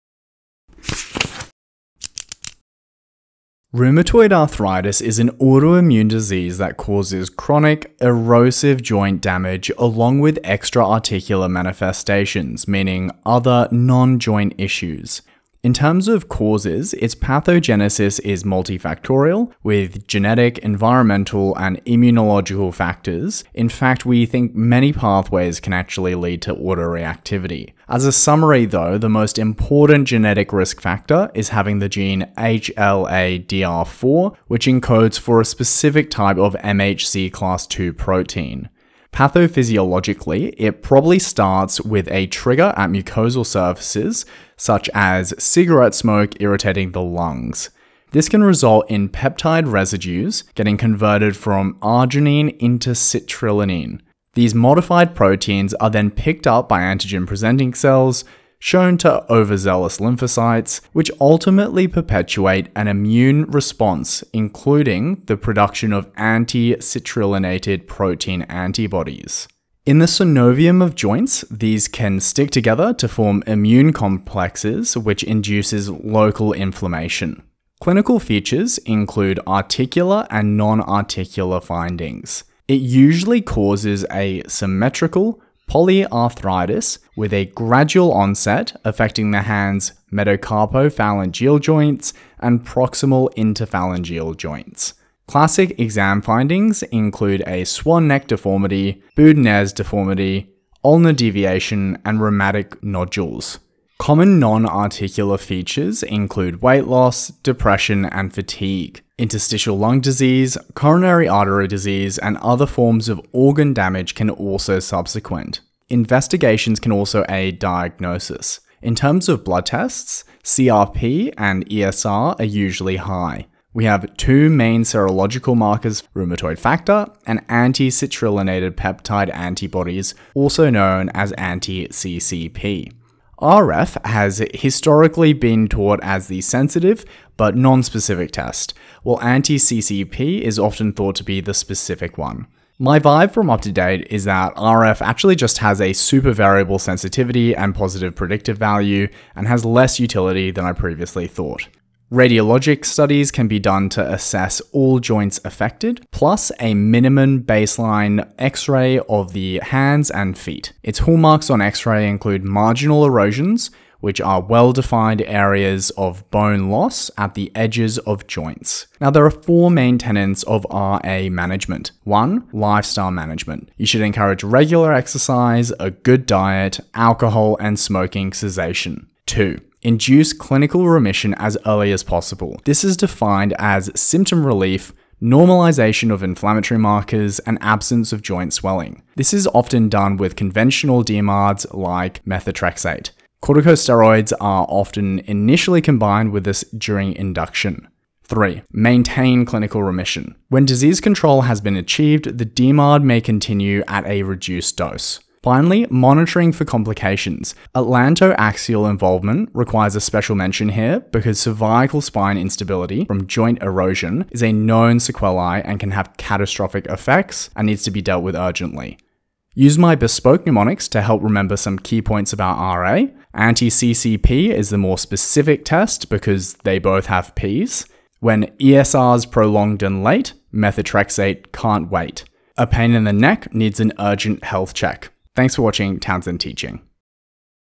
Rheumatoid Arthritis in 3 Minutes
Rheumatoid Arthritis pathogenesis, clinical features, diagnosis, and management with mnemonics for medical students and nursing students.

Rapid review of the disease with a clinical focus.

0:00 Introduction
0:15 Pathogenesis
1:17 Clinical Features
1:55 Investigations
2:50 Management
3:40 Mnemonic